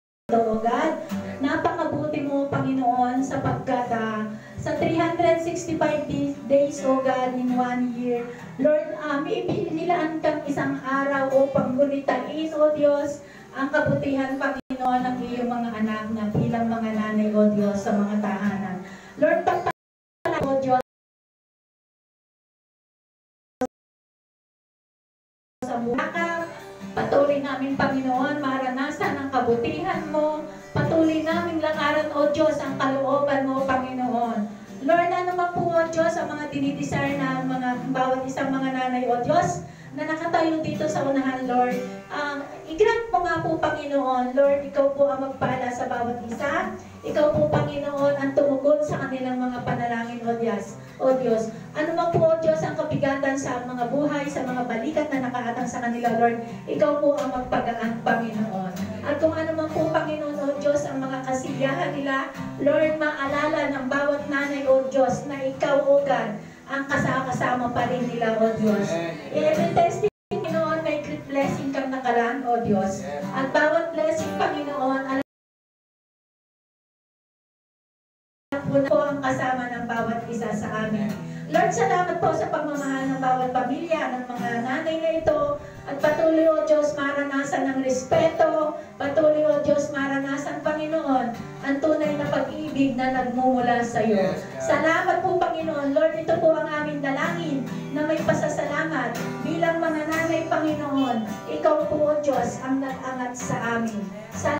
MOTHERS'DAY AMEN TO PRAYERS
Celebrating Mothers Day Special. Prayers for all Moms by Ptra. Eddie Fradejas.

God bless!